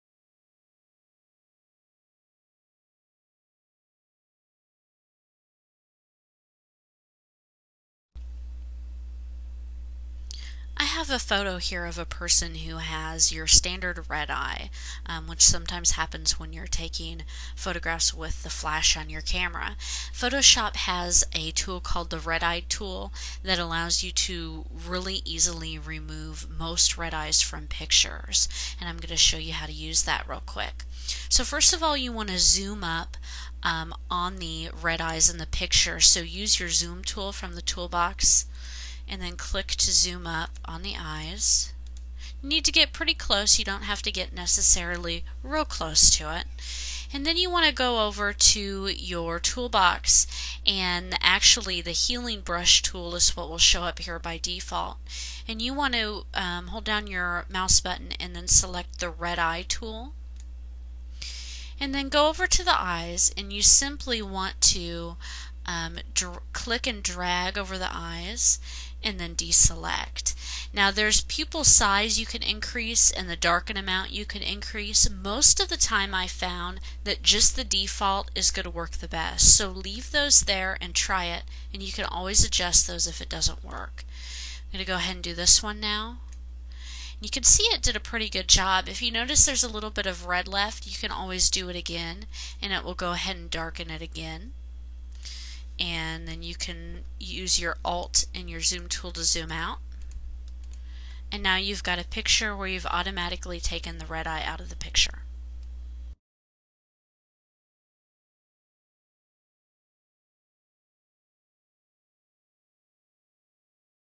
Remove Red Eye in Photoshop CS3
See a higher quality video on TechTutor.TV! You can easily remove red eye from photos using the Adobe Photoshop CS3 red eye tool. In this video tutorial, you will learn how to remove red eye in Photoshop CS3. See higher quality tutorials all for FREE